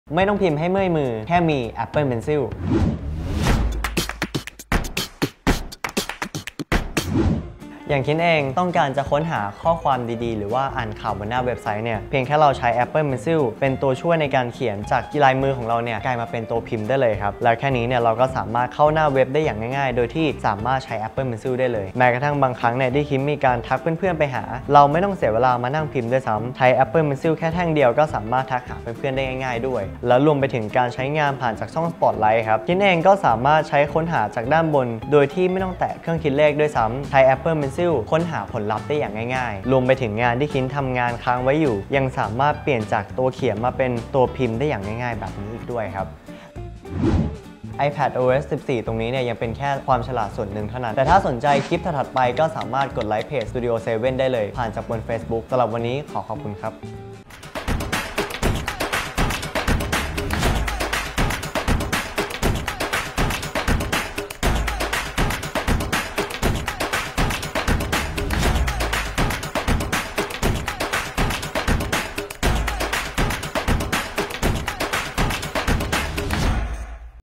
วิธีใช้ Apple Pencil แทนการพิมพ์ | Tips & Tricks by Studio 7
รู้หรือไม่ ว่าเราสามารถใช้ Apple Pencil ขีดเขียนแทนการพิมพ์ได้แล้ว Studio 7 Tips & Tricks คลิปนี้มีวิธีการทำแบบง่ายๆมาฝาก

สนใจสินค้า Apple ราคาพิเศษ